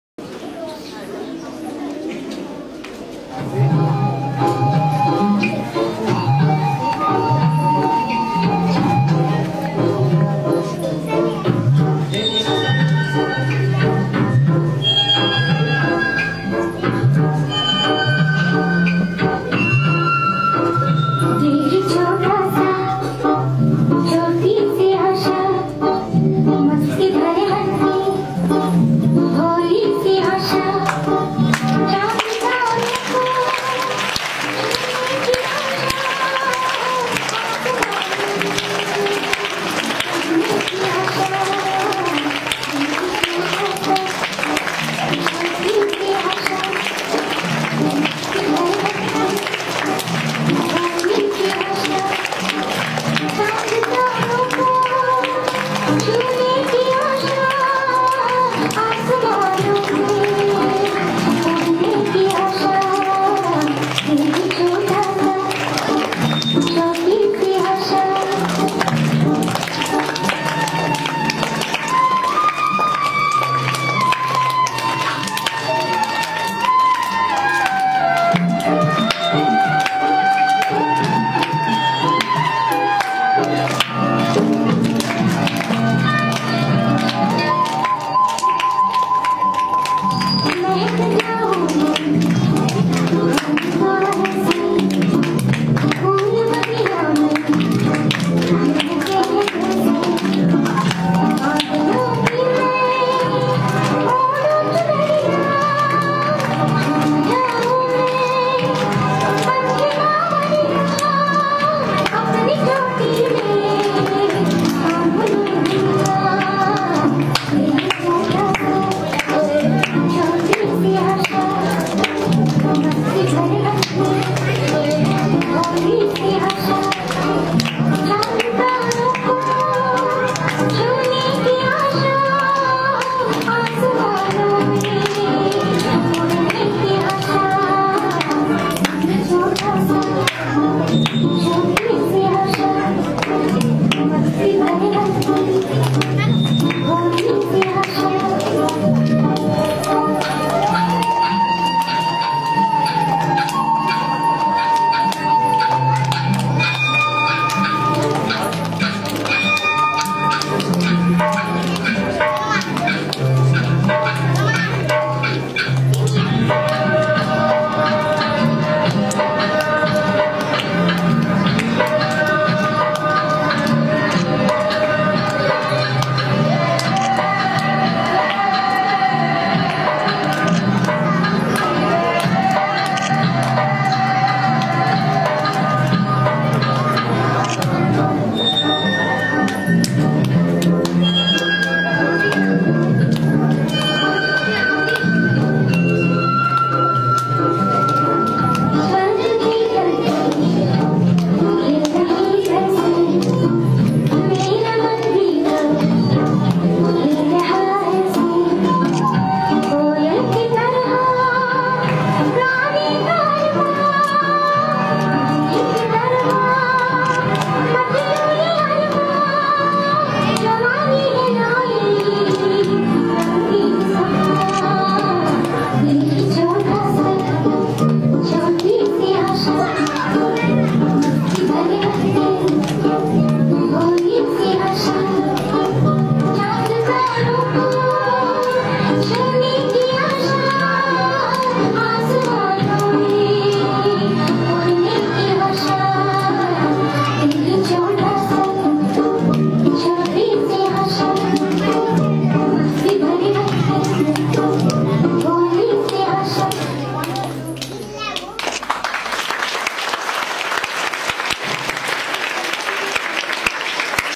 Children's day celebration
students of Bal Niketan residential school performing on the occasion of children's day.